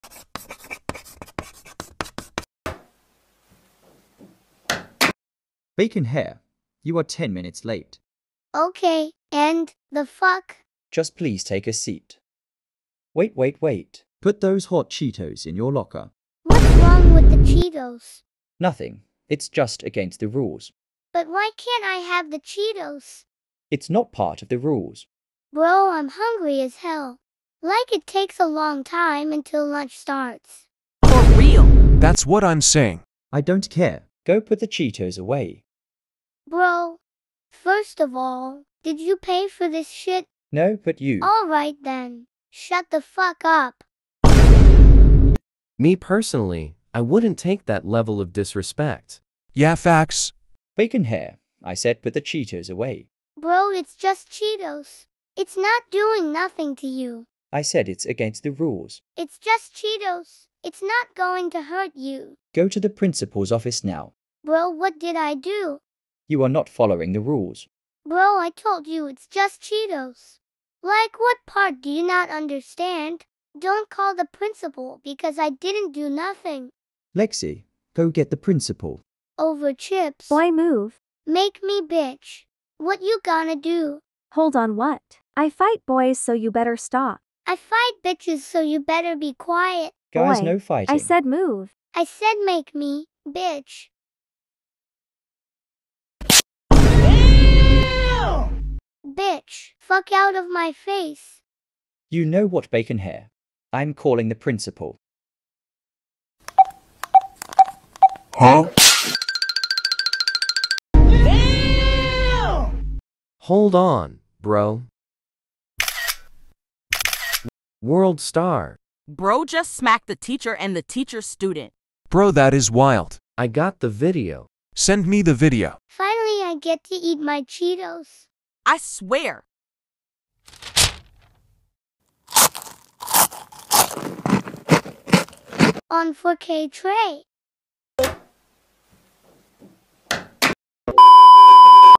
When Bacon Hair comes to class with hot Cheetos
This shows when bacon hair comes to class with hot Cheetos

WARNING: A little Violence, and swearing￼